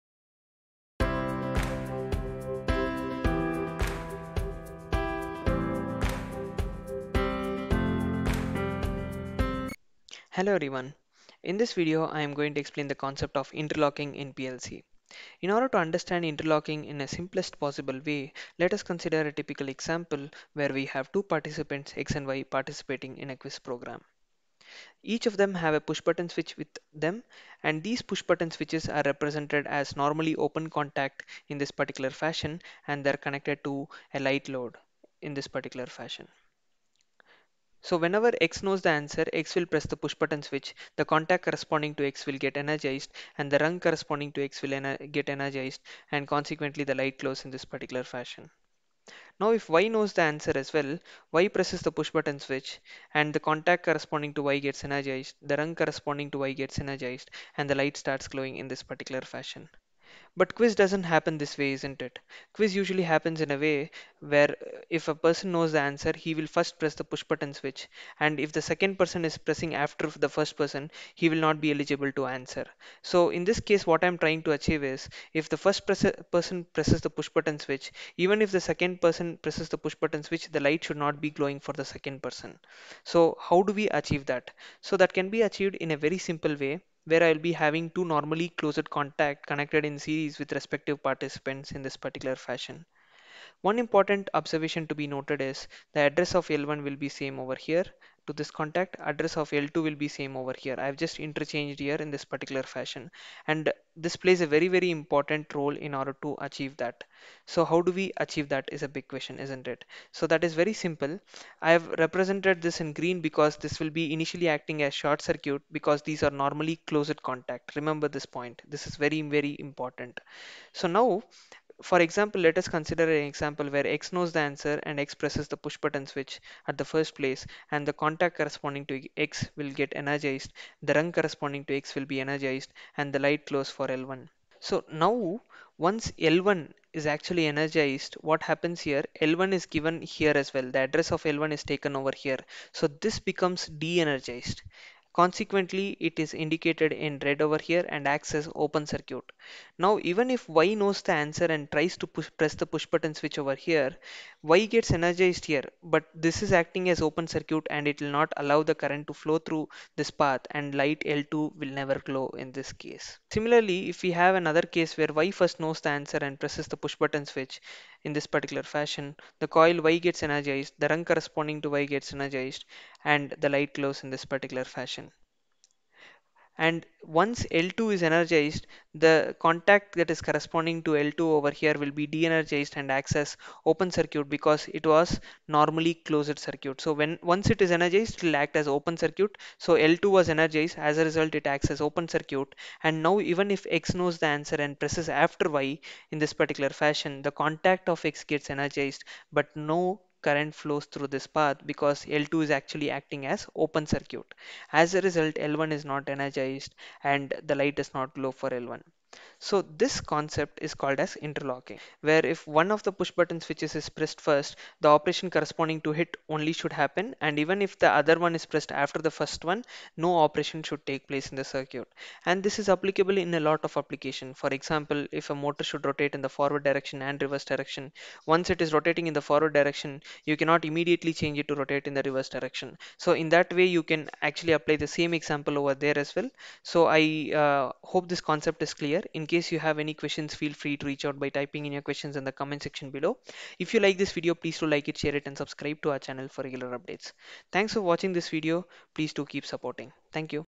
Interlocking in PLC
This video demonstartes the concept of Interlocking in PLC.